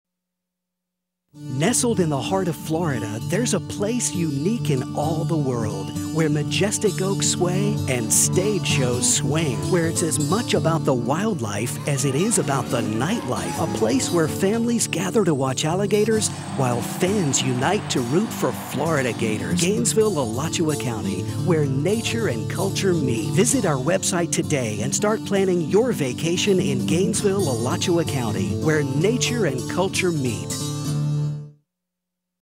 Alachua County Tourism "Where Nature and Culture Meet" TV Commercial
Alachua County offers tourists a truly unique blend of natural attractions and cultural events. We compared and contrasted that dichotomy while leveraging a poetic tone to inspire new visitors to check out all Gainesville and Alachua County have to share.